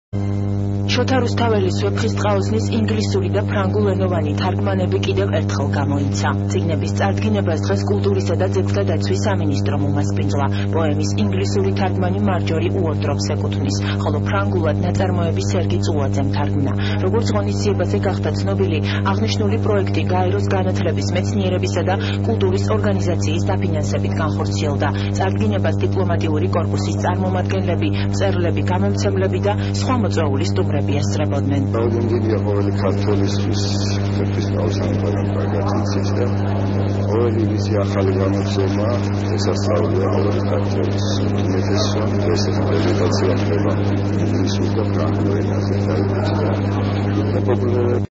''ვეფხისტყაოსნის'' ფრანგული და ინგლისურენოვანი გამოცემების პრეზენტაცია
18 სექტემბერს, საქართველოს კულტურისა და ძეგლთა დაცვის სამინისტროში, „ვეფხისტყაოსნის" პირველი სტამბური გამოცემის 300 წლისთავთან დაკავშირებით, გაიმართა ინგლისურ და ფრანგულენოვანი "ვეფხისტყაოსნის" პრეზენტაცია.

აღნიშნული გამოცემები საქართველოს კულტურისა და ძეგლთა დაცვის სამინისტროსა და გაეროს განათლების, მეცნიერებისა და კულტურის ორგანიზაციის (UNESCO) დაფინანსებით, UNESCO-ს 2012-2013 წლების „მონაწილეობის პროგრამის" ფარგლებში განხორციელდა.

„ვეფხისტყაოსნის" პირველი ბეჭდური გამოცემის 300 წლისთავი იუნესკოს 36-ე გენერალური კონფერენციის გადაწყვეტილებით იუნესკოს მონაწილეობით აღინიშნება.

„ვეფხისტყაოსნის" ინგლისური თარგმანი ეკუთვნის მარჯორი უორდროპს, ხოლო ფრანგული სერგი წულაძეს. ორივე გამოცემა დასურათებულია ს. ქობულაძის მიერ შესრულებული ილუსტრაციებით.

თარგმანები გამოსცა ბაკურ სულაკაურის გამომცემლობამ. გამოცემას ორგანიზებას უწევდა სსიპ. გიორგი ჩუბინაშვილის სახელობის ქართული ხელოვნების ისტორიისა და ძეგლთა დაცვის ეროვნული კვლევის ცენტრი, საქართველოს იუნესკოს საქმეთა ეროვნული კომისიის მხარდაჭერით.

ღონისძიებას დაესწრენენ საქართველოს კულტურისა და ძეგლთა დაცვის მინისტრის მოადგილე ბადრი ბაგრატიონ-გრუზინსკი, დიპლომატიური კორპუსისი წარმომადგენლები, მწერლები, გამომცემლები, მოწვეული სტუმრები.